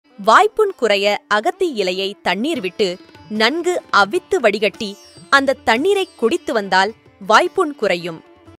வாய்ப்புண் குறைய
அகத்தி இலையை தண்ணீர் விட்டு நன்கு அவித்து வடிகட்டி அந்த தண்ணீரை குடித்து வந்தால் வாய்ப்புண் குறையும். @selviyinselvan